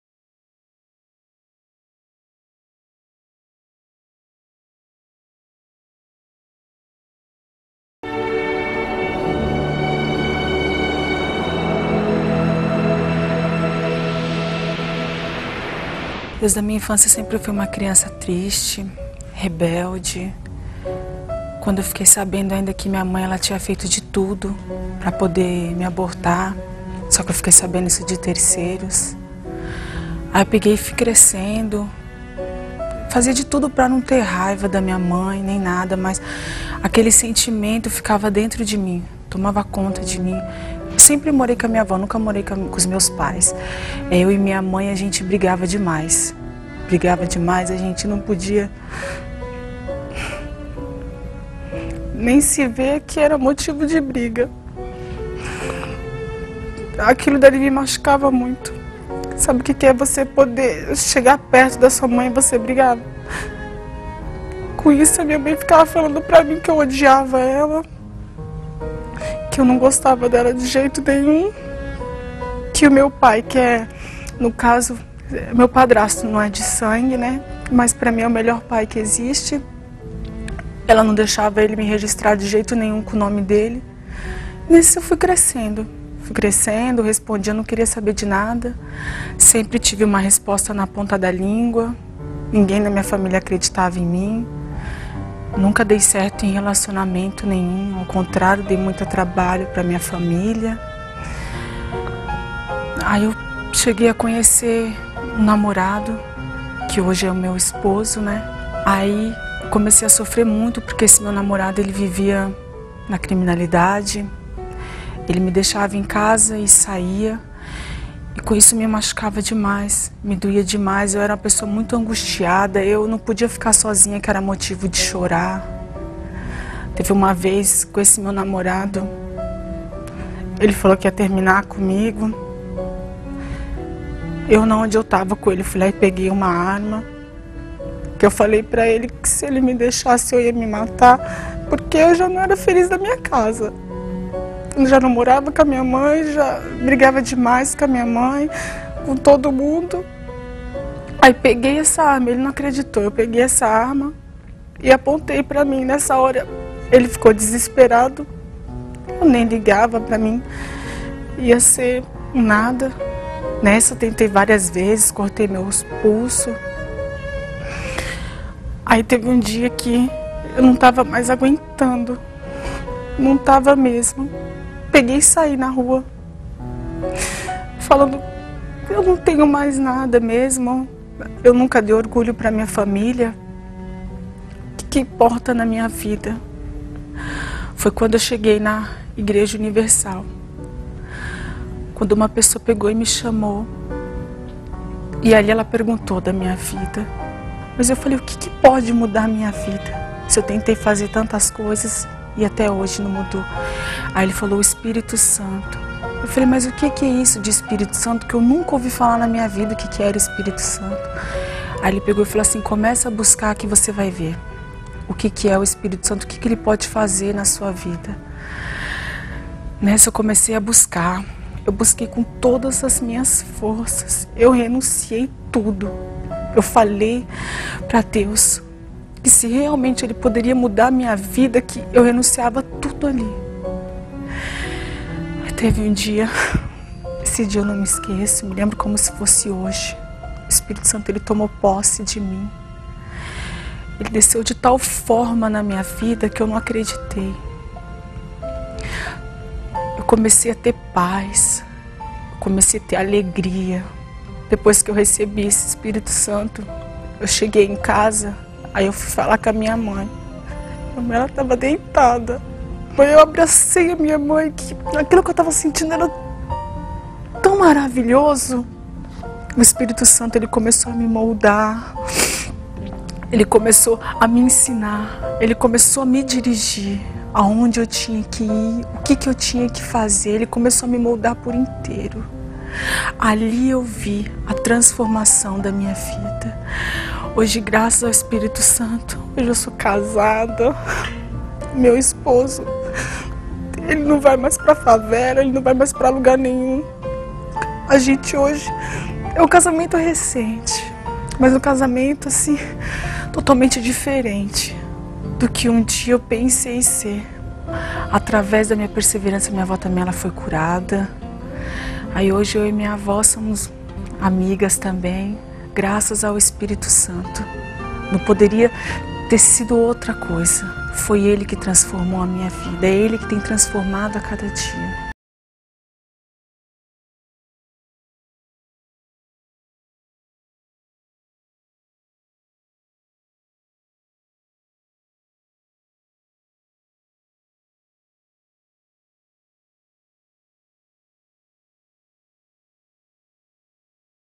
Nem minha família acreditava em mim | Igreja Universal
Nem a família dela acreditava mais, mas Deus sempre esteve com ela.